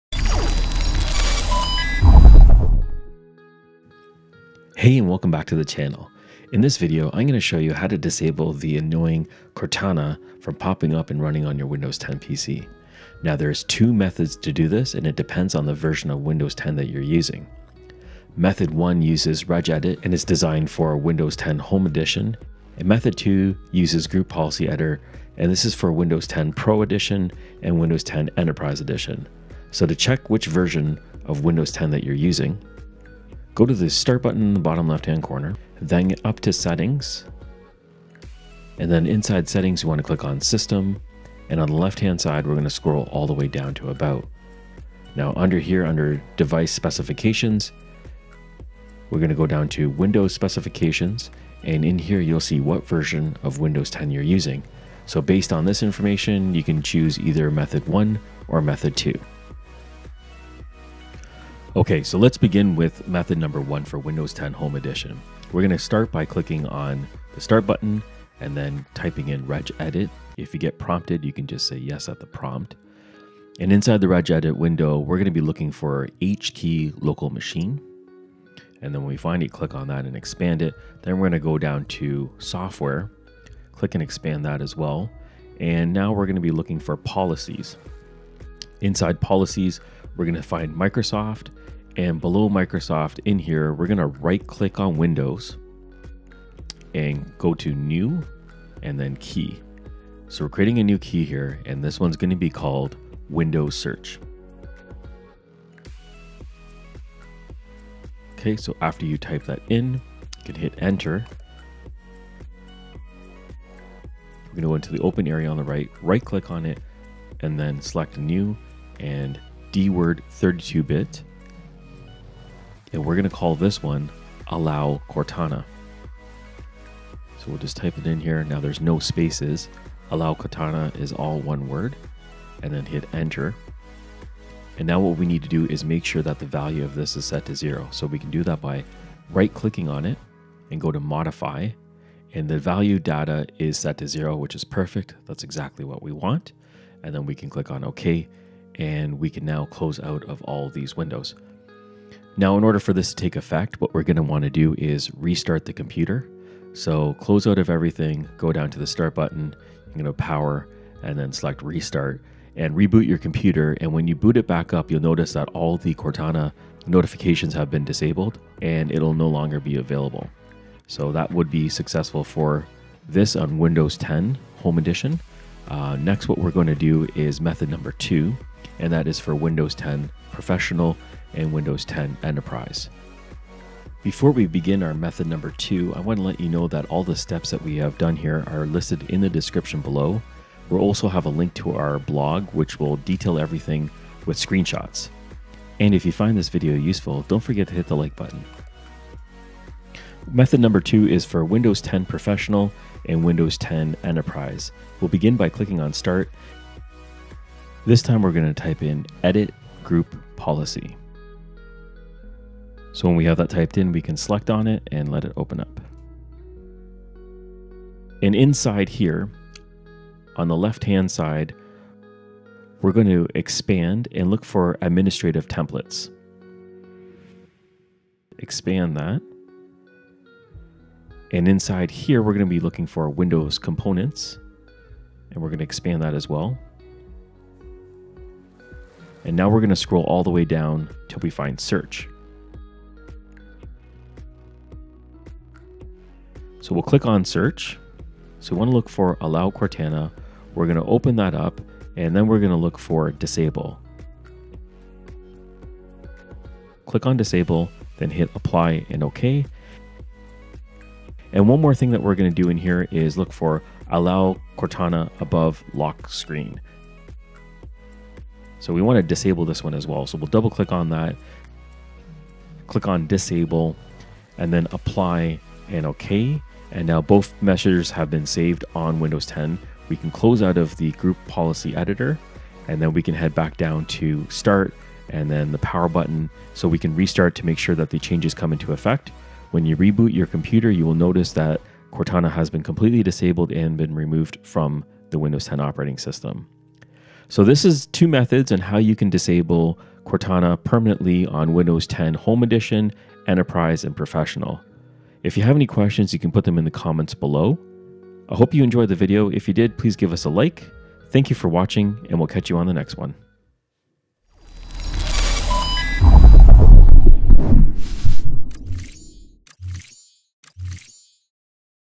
How to Disable Cortana Windows 10 Permanently! | How To Turn Off Cortana Windows 10
In this video, I will show you how to permanently disable and how to turn off Cortana windows 10.  There is two methods and it depends on the version of windows 10 that you are using. Method one is for Windows Home edition, and Method 2 is for Windows Professional, and Enterprise addition.

Time Stamps:
0:00 Introduction
0:32 Finding your version of Windows
1:02 Method 1 - Windows Home
3:15 Method 2 - Windows Professional & Enterprise

Video Notes:

Method 1 - Windows 10 Home
Inside the Registry Editor, click and expand HKEY LOCAL MACHINE/ /SOFTWARE/ Policies/ Microsoft/ Windows
Right click and make a new key called 'Windows Search'
On the right side of the window, right-click and make a New/DWORD 32bit entry. Call it 'AllowCortana' and hit the enter key. Right-click on it and select 'Modify...' and  make sure the Value Data is set to 0.
Close out of the windows and restart computer for changes to take effect.

Method 2 - Windows 10 Professional and Enterprise
Open the Group Policy Editor (gpedit)
Navigate to  Local Computer Policy/Computer Configuration/Administrative Templates.
Below expand 'Windows Components' then select 'Search'
On the right side, look for the setting called 'Allow Cortana.' open it, then select disable.
Next, look for 'Allow Cortana above lock screen' open it, then select disable.